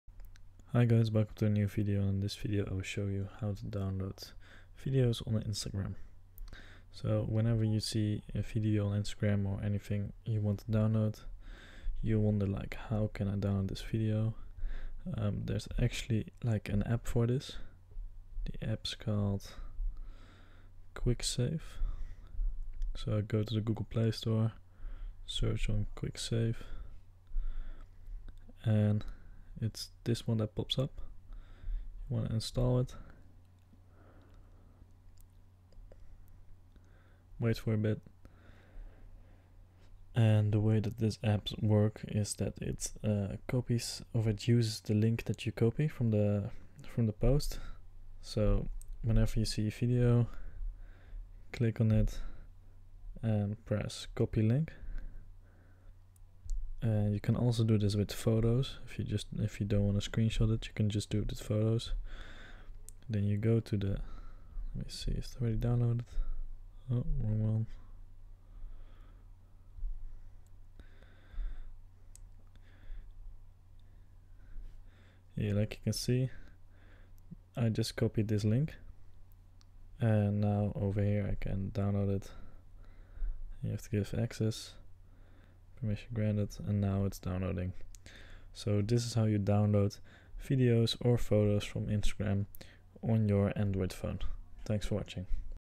How To Download Instagram Videos on Android 2020
How To Download Instagram Videos on Android
In this video I will show you how to download Instagram videos on android